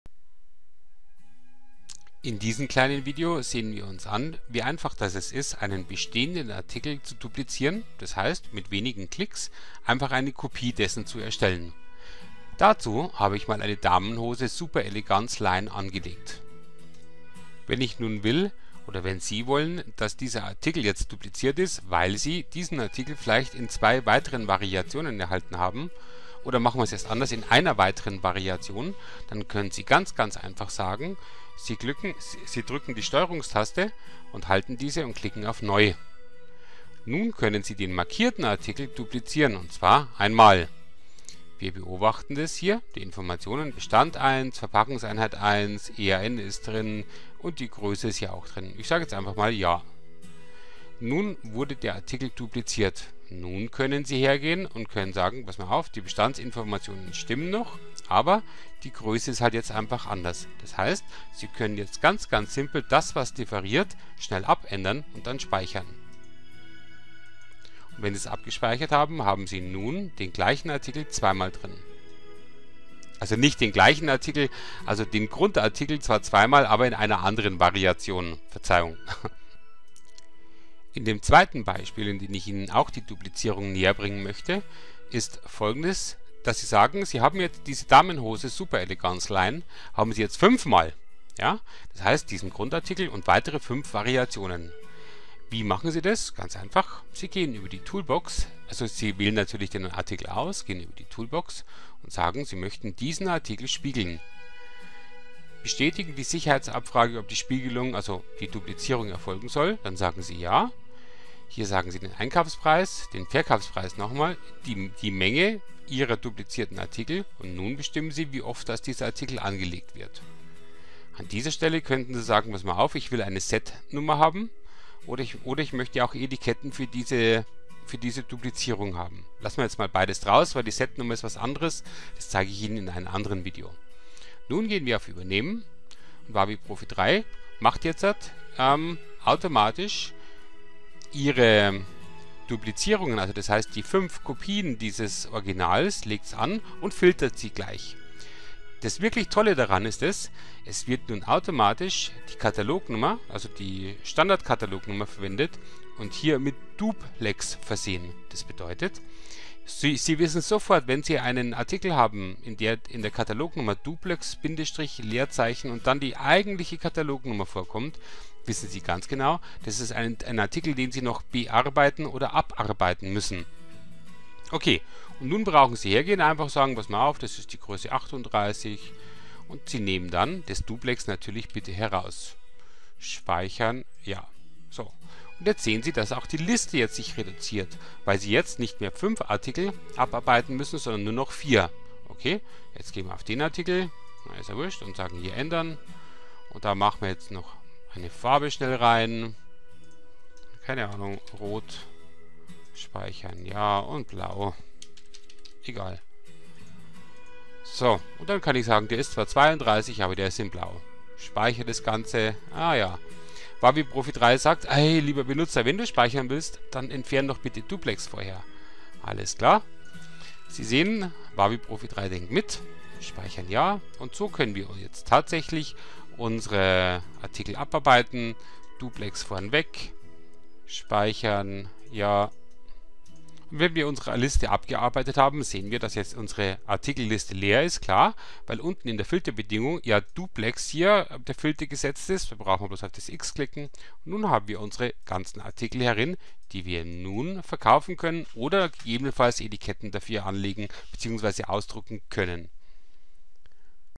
WaWi-Profi 3.x, Artikelstamm, Artikel dublizieren
So erstellen Sie sehr schnell einzelne, oder mehrere "Duplikate" es Artikels!